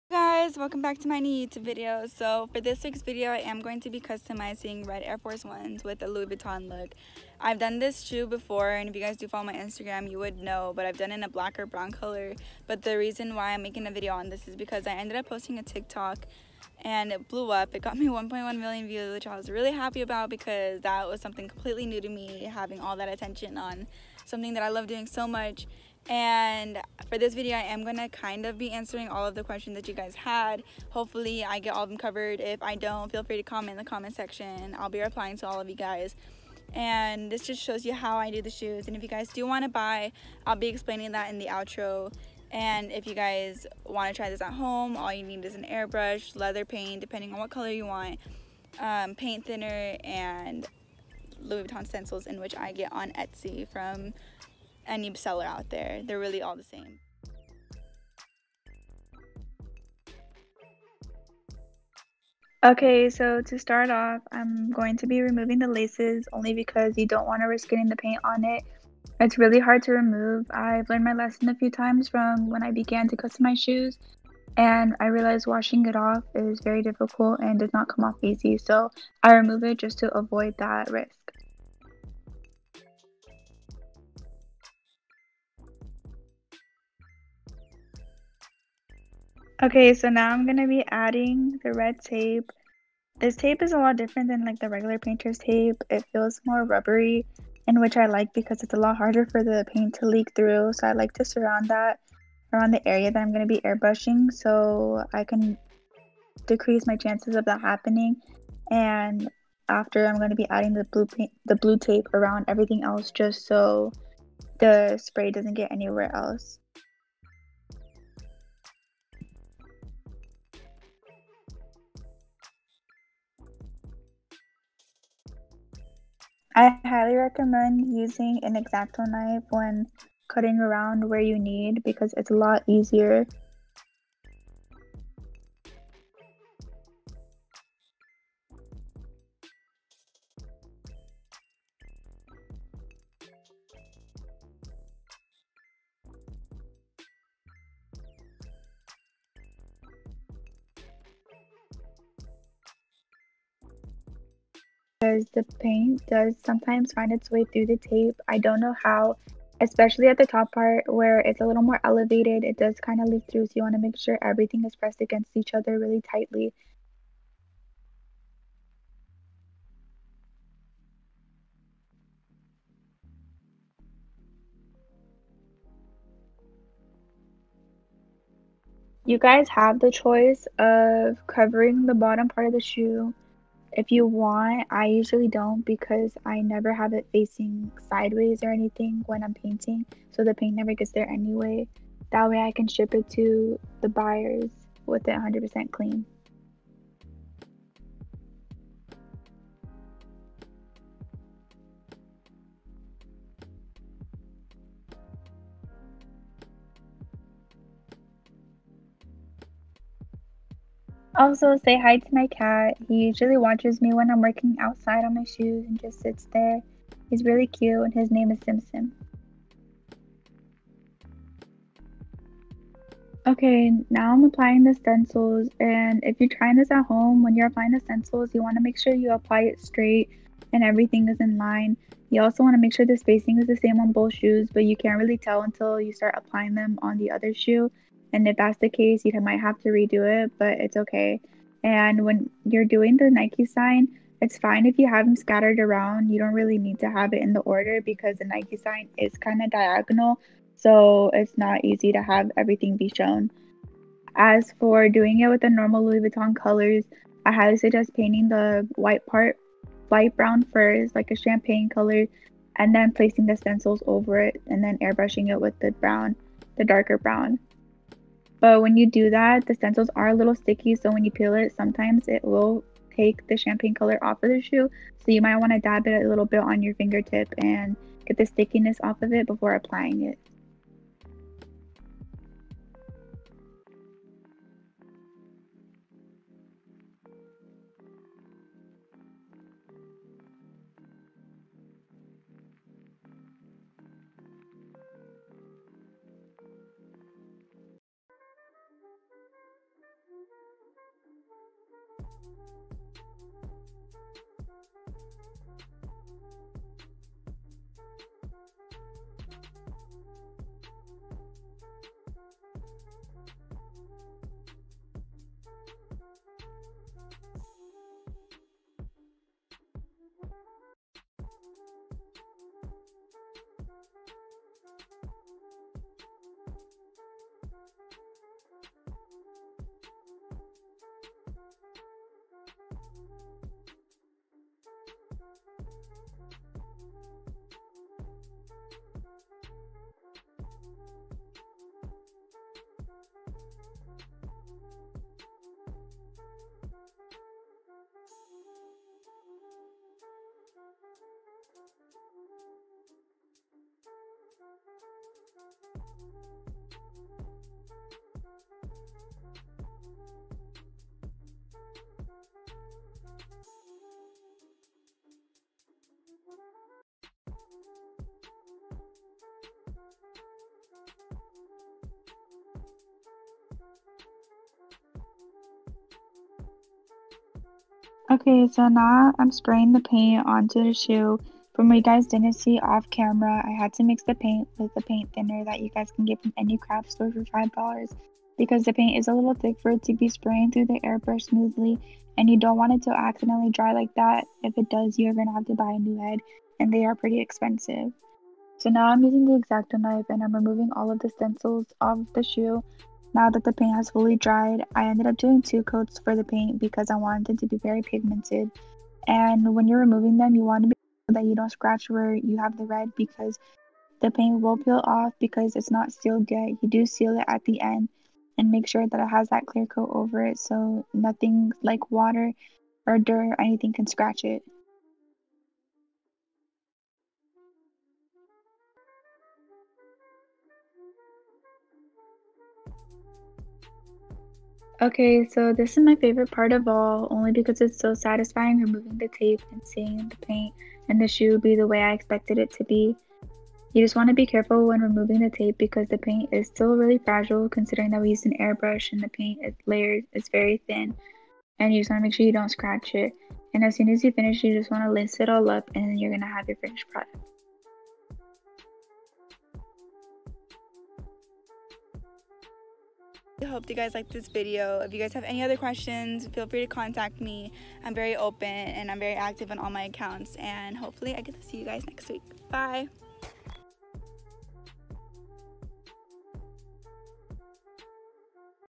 Red Louis Vuitton Custom AF1 Tutorial
What’s up guys! Here is my little tutorial on customizing air forces. If you have any suggestions for designs, let me know! Website coming soon!